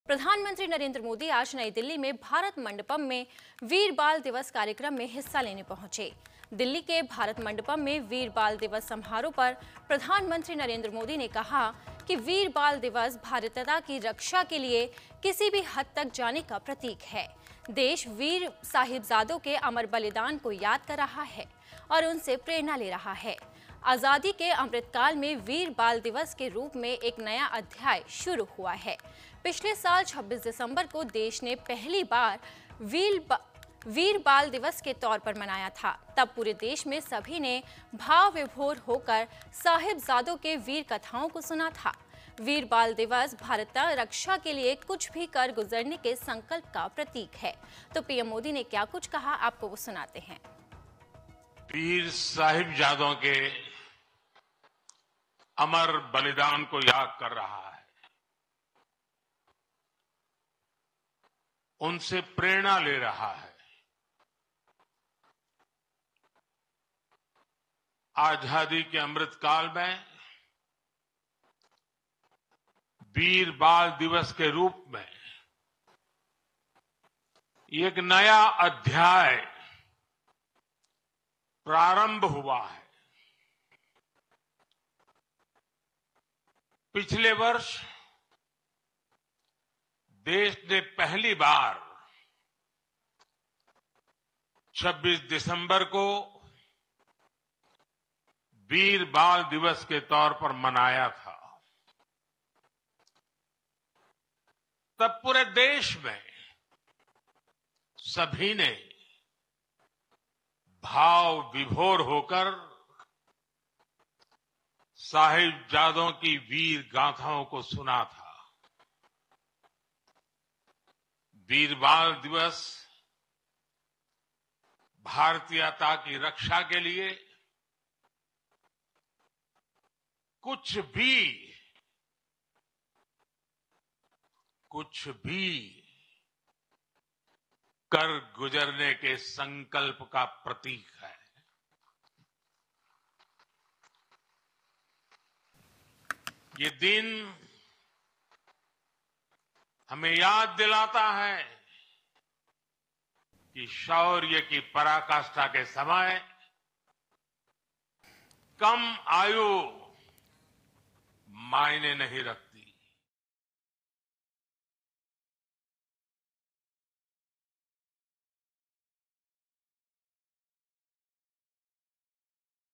Veer Bal Diwas 2023 : वीर बाल दिवस पर PM Modi का खास संदेश
प्रधानमंत्री नरेंद्र मोदी आज नई दिल्‍ली में भारत मंडपम में वीर बाल दिवस कार्यक्रम में हिस्‍सा लिया। दिल्ली के भारत मंडपम में 'वीर बाल दिवस' समारोह पर प्रधानमंत्री नरेंद्र मोदी ने कहा, "...'वीर बाल दिवस' भारतीयता की रक्षा के लिए किसी भी हद तक जाने का प्रतीक है... देश वीर साहिबजादों के अमर बलिदान को याद कर रहा है और उनसे प्रेरणा ले रहा है। आजादी के अमृतकाल में वीर बाल दिवस के रूप में एक नया अध्याय प्रारंभ हुआ है। पिछले वर्ष 26 दिसंबर को देश ने पहली बार 26 दिसंबर को वीर बाल दिवस के तौर पर मनाया था तब पूरे देश में सभी ने भाव विभोर होकर साहिबजादों के वीर कथाओं को सुना था। वीर बाल दिवस भारतीयता रक्षा के लिए कुछ भी कर गुजरने के संकल्प का प्रतीक है।